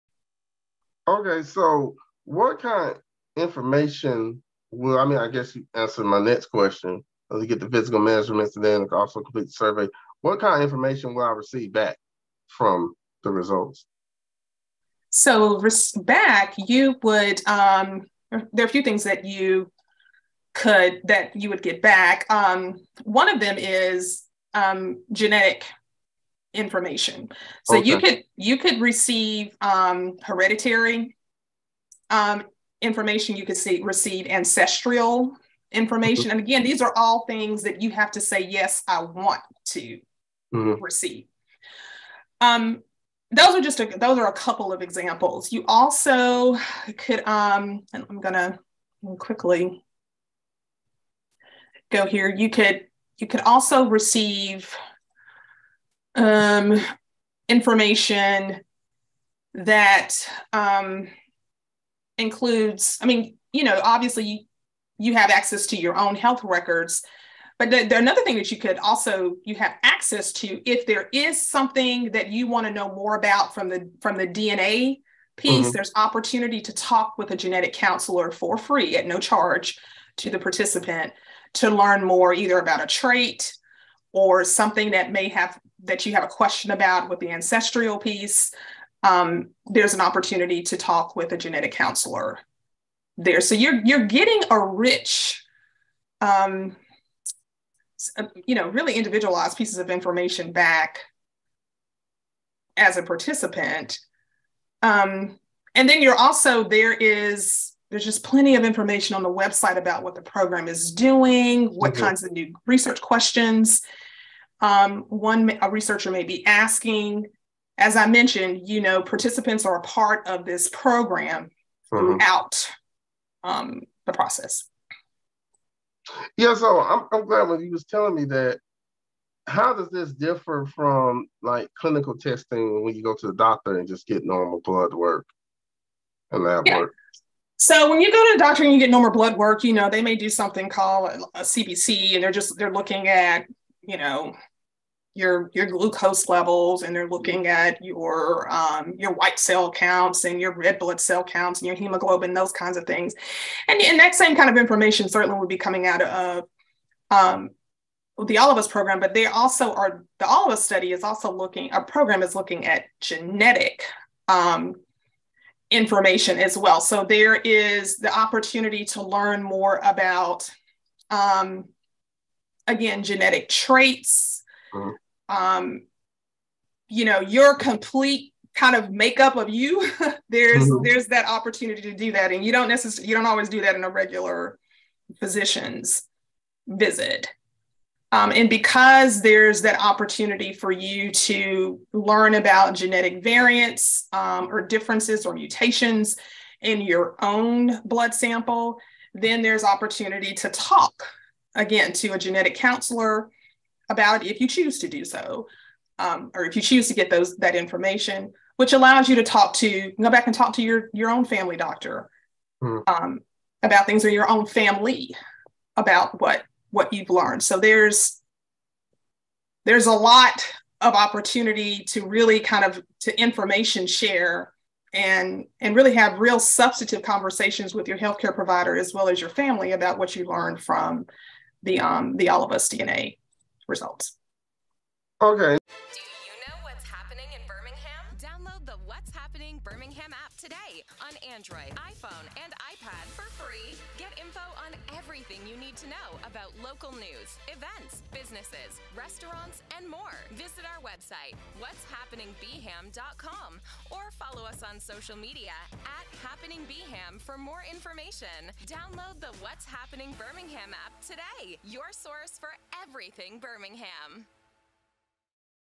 What kind of health information will I receive from participating in the All of Us Research Program?
Dr. Shauntice Allen on the type of health information you will receive from participating in the All of Us Research Program. Visit allofus.uabmedicine.org for more information and to join.